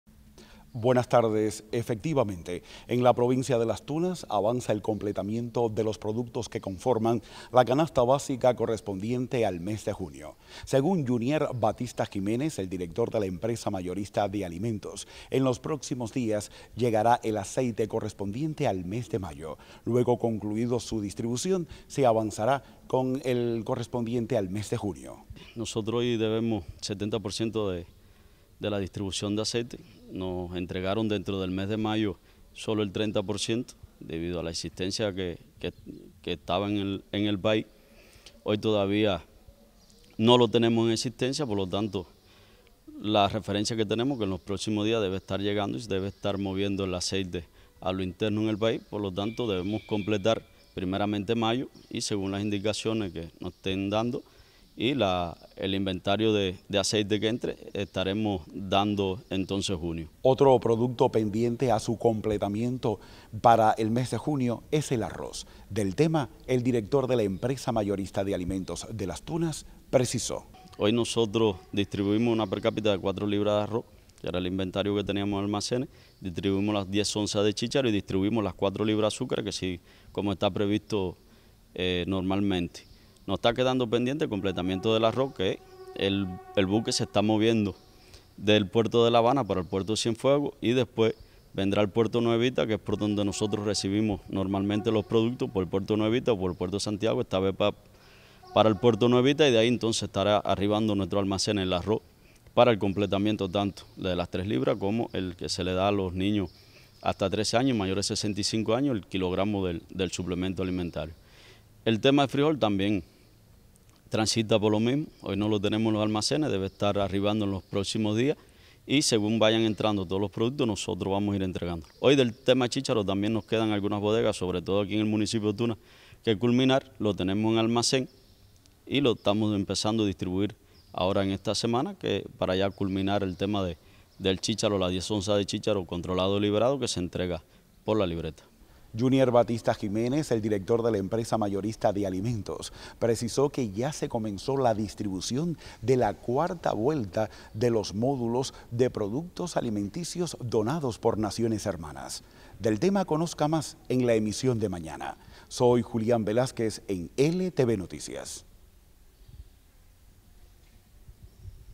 Anuncian en LasTunas distribución de productos de la canasta básica en los próximos días
Las Tunas avanza en el completamiento de la canasta básica del mes de junio. El periodista Julián Velázquez nos informa sobre la distribución de aceite en los próximos días.